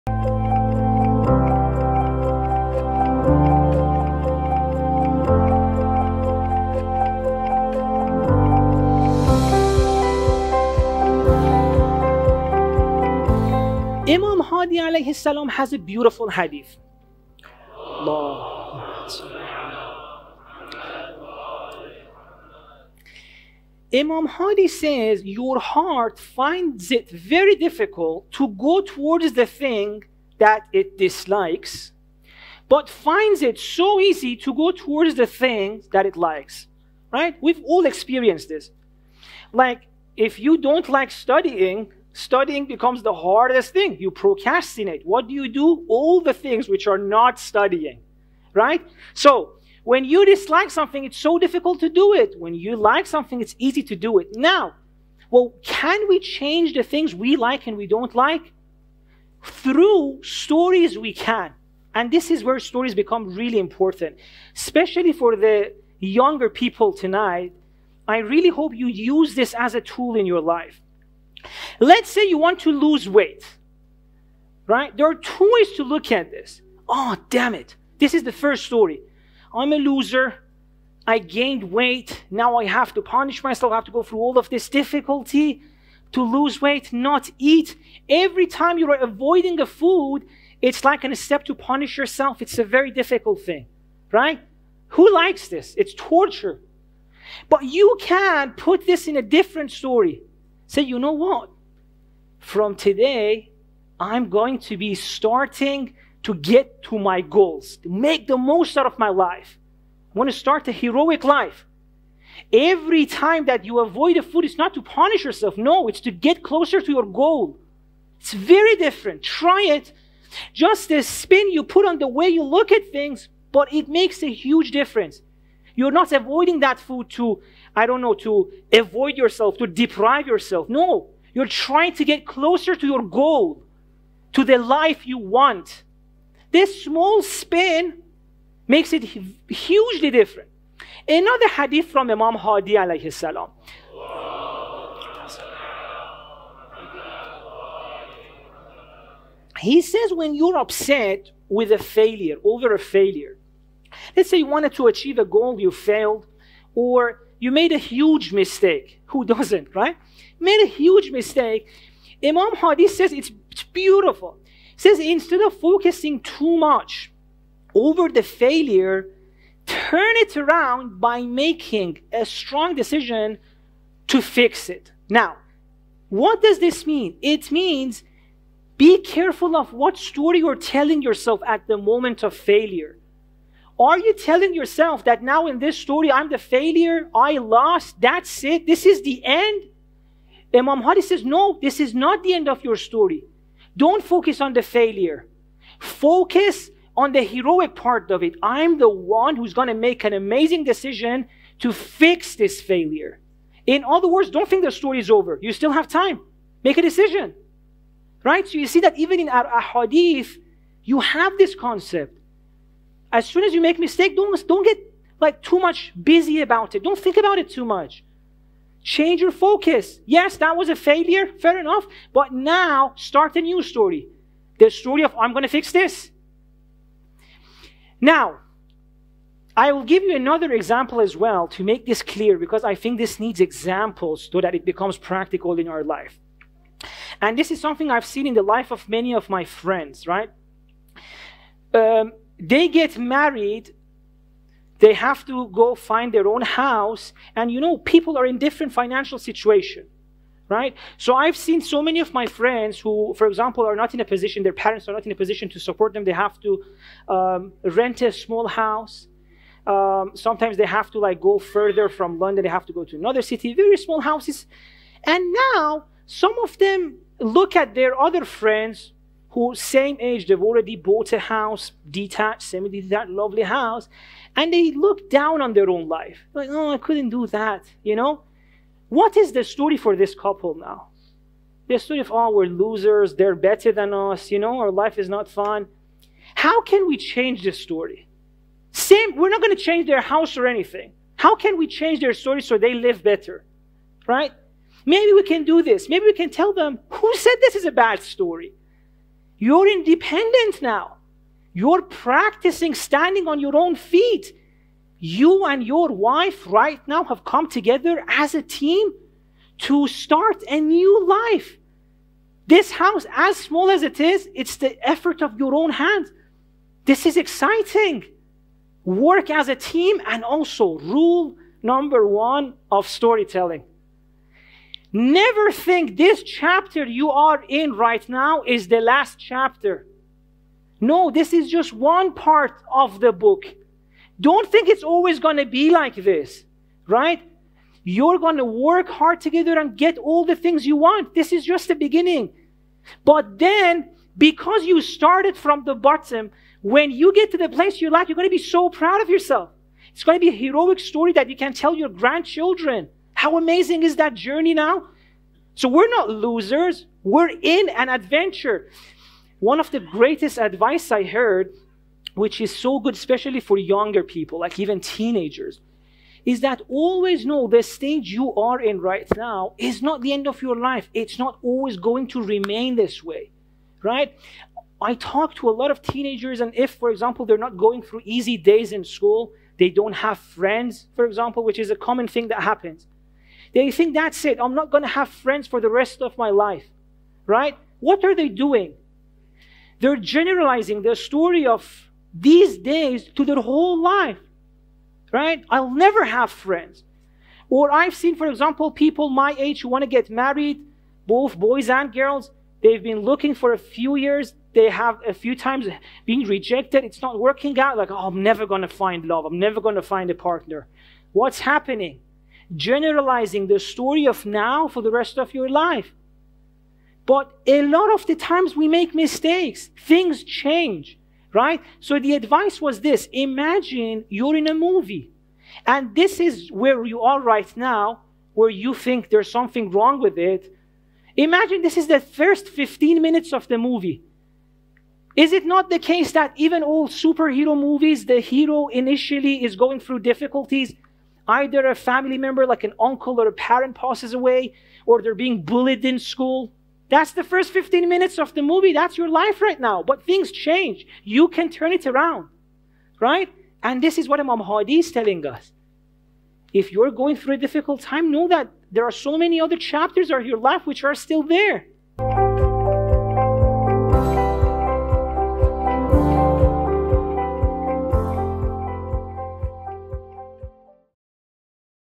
DAY 89: Change The Way You Look At Challenges | Sheikh Javad Shomali
We all face challenges as part of our life. There are goals we wish to achieve for e.g losing weight or problems we are facing for e.g financial issues. And in these adversaries, we often look down upon ourselves and compare ourselves to those who have achieved. So is there a way to get out of this hopelessness?

In this 100Days100Clips, Sheikh Javad Shomali shares an enlightening perspective on our such challenges through hadiths of Imam Hadi (alaihis salaam).